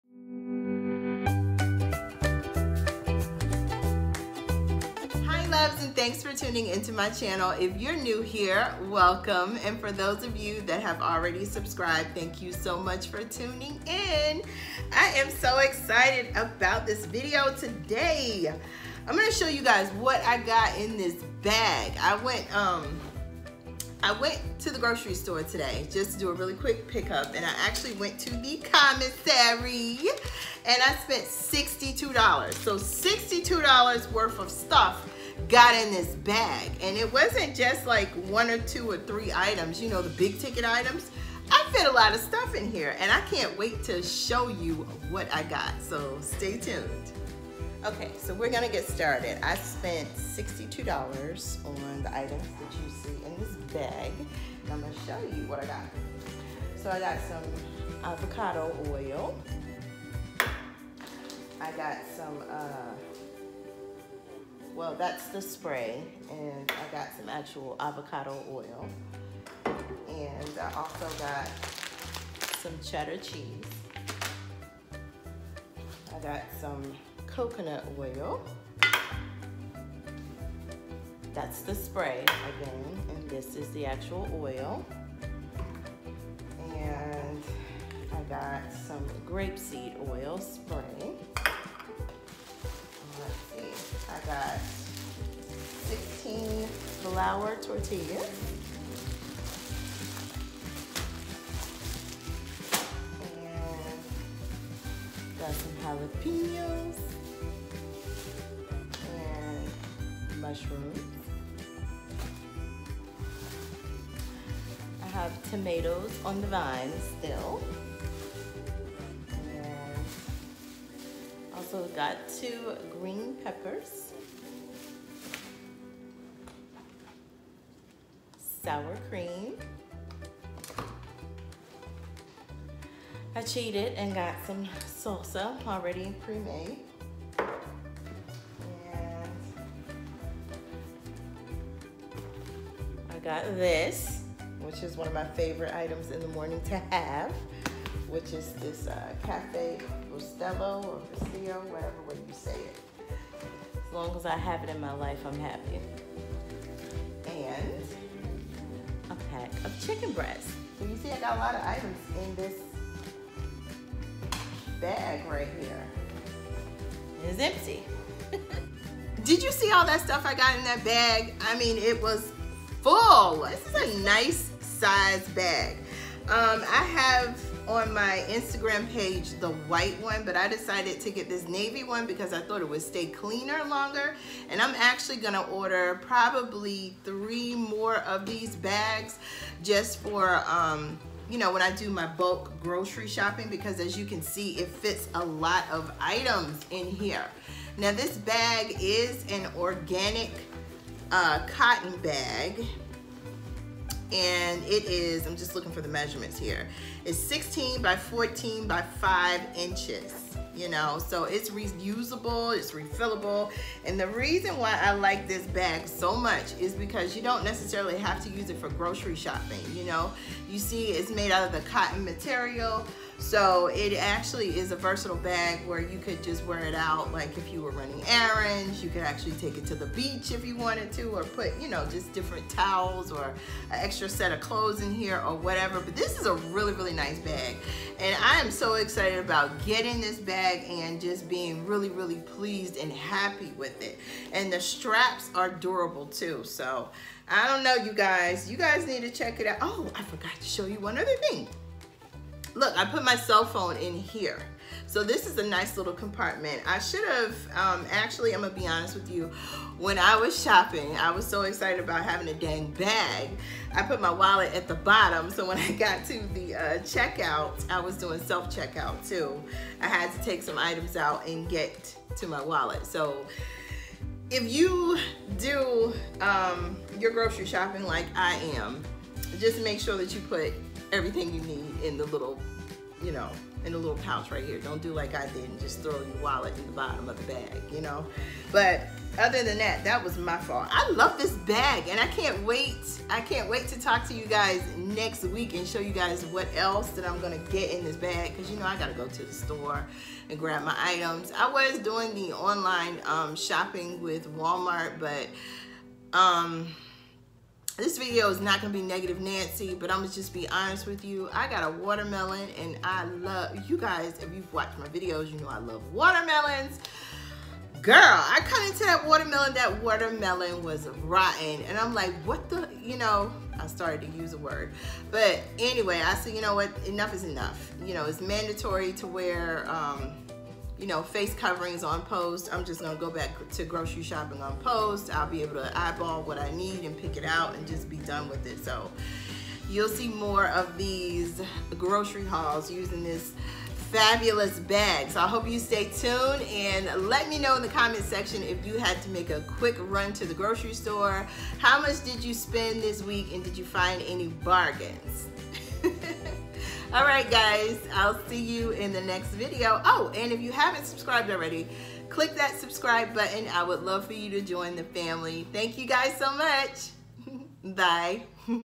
COMMISSARY HAUL/ $62 WORTH OF STUFF WAS IN THIS BAG/MILITARY GROCERY STORE SHOPPING WITH GROVE BAG
Mrs. Meyer's Dishwasher Tabs (20 ct)

T W I T T E R - eyeshadowshopp1

CHECK OUT MY FIRST IMPRESSIONS ON PRODUCTS I PURCHASED FROM GROVE